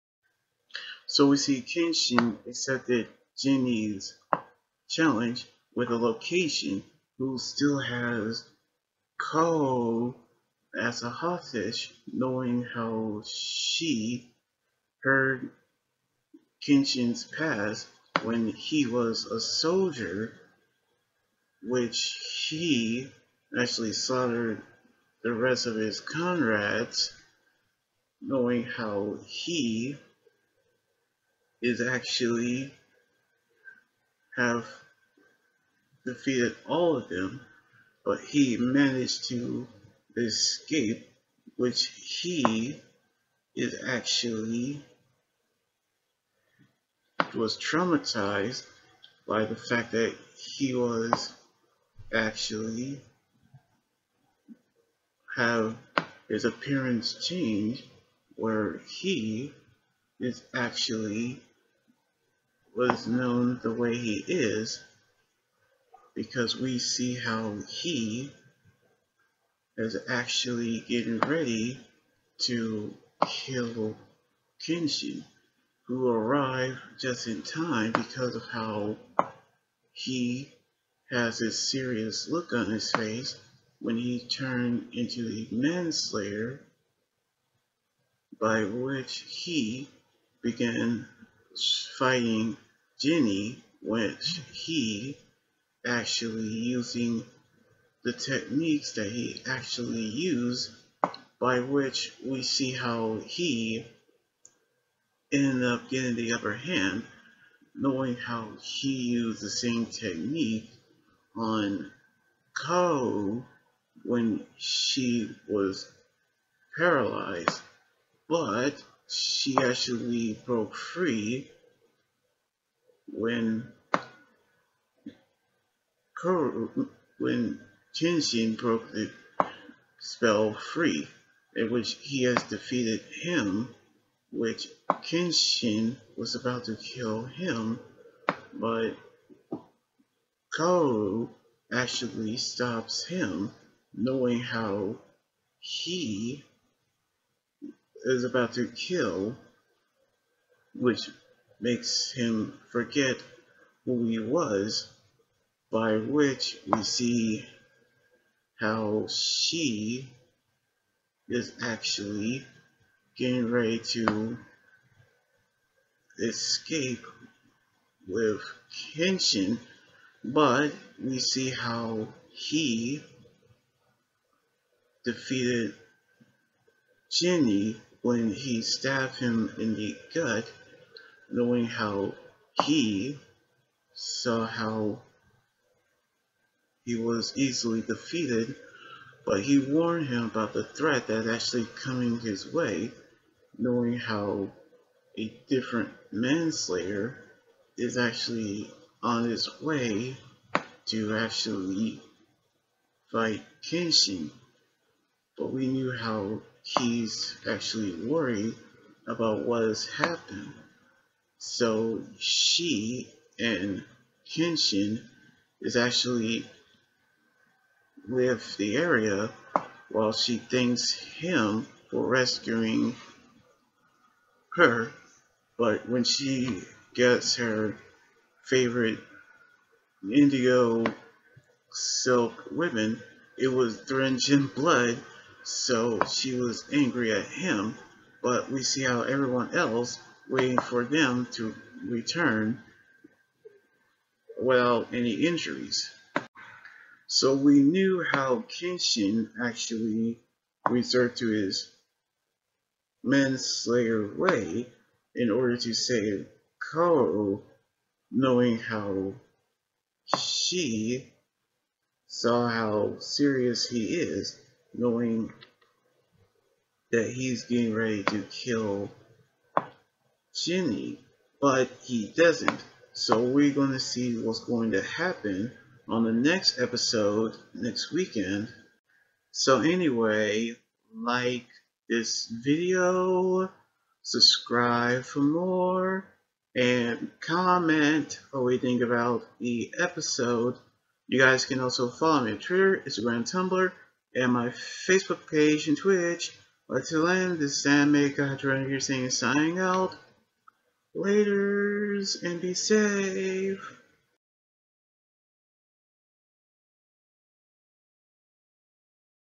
Rurouni Kenshin Episode 7 Review. Kenshin Vs. Jine
Kenshin battles jine to save kaoru.

Outro and thumbnail made in placeit.net